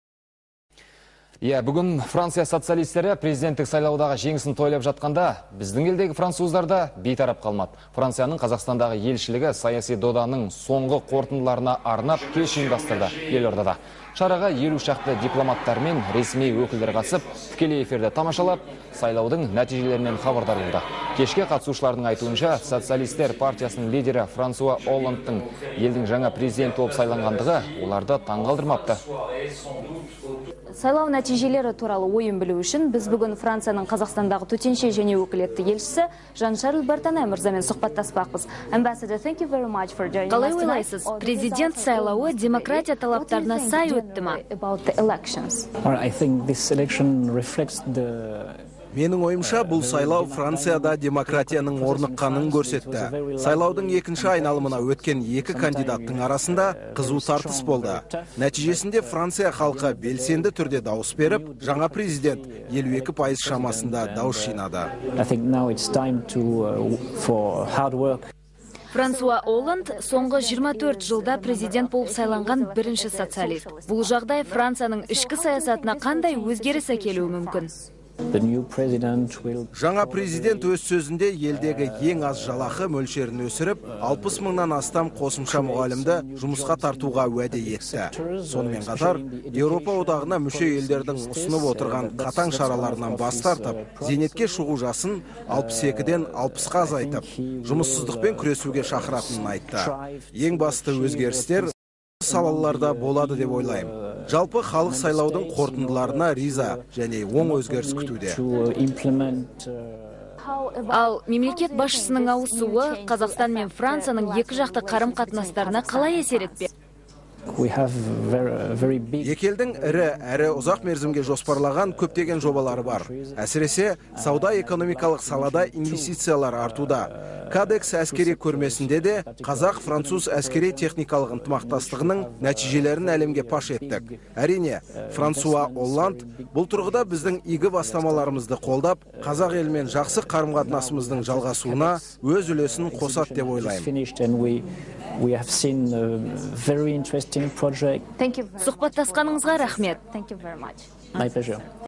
Франсуа Олланд президенттік сайлауда жеңіске жетті
Франция социалистері президенттік сайлаудағы жеңісін тойлап жатқанда, еліміздегі француздар да қол қусырып отырған жоқ.